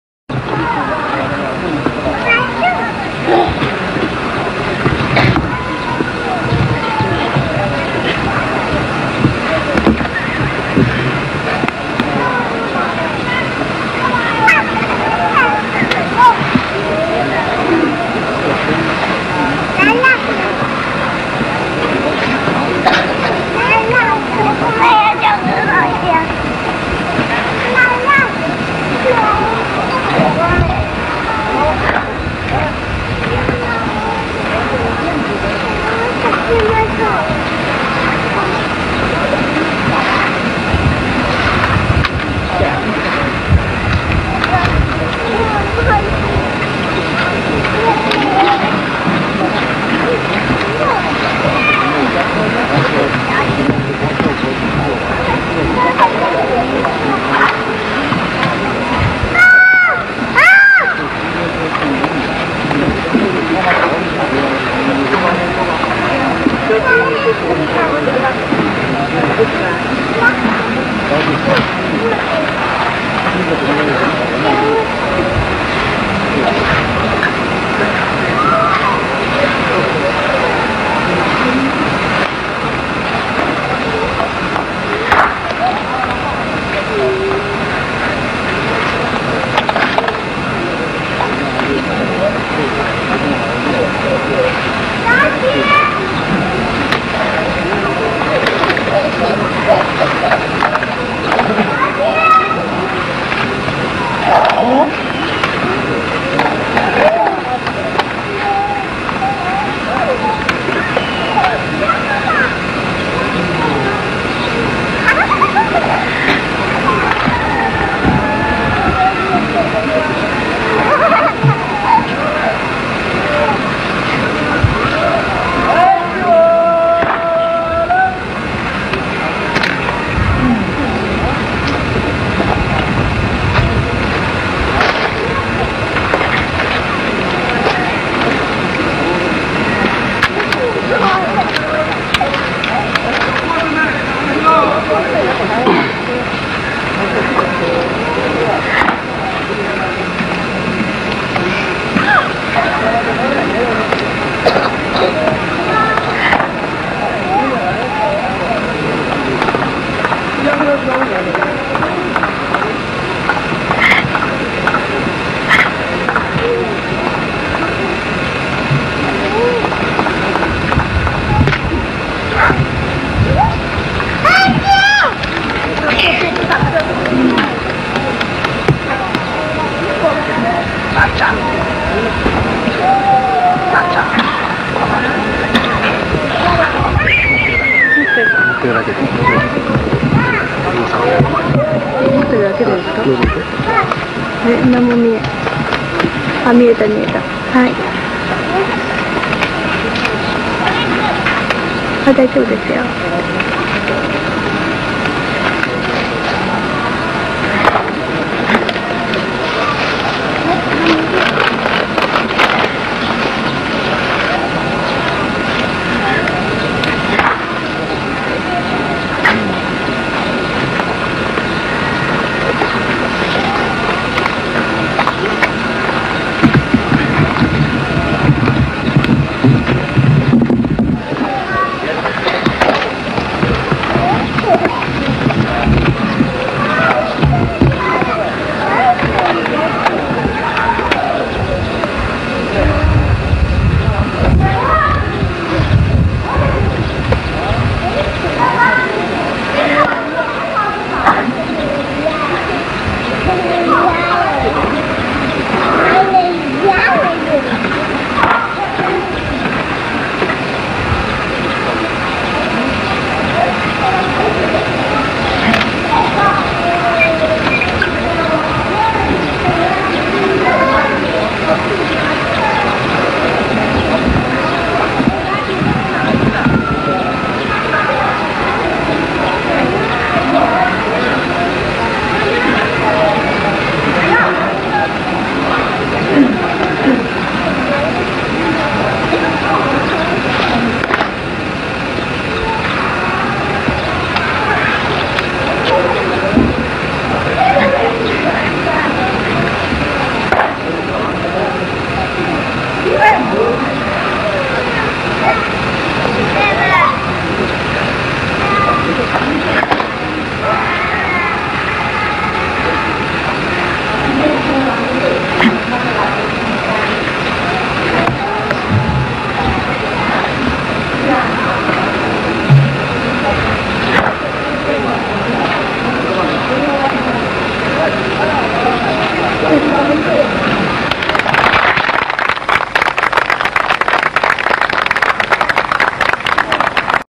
chen xiaoxing xinjia 1984
A young Chen Xiaoxing performs xinjia Yilu in 1984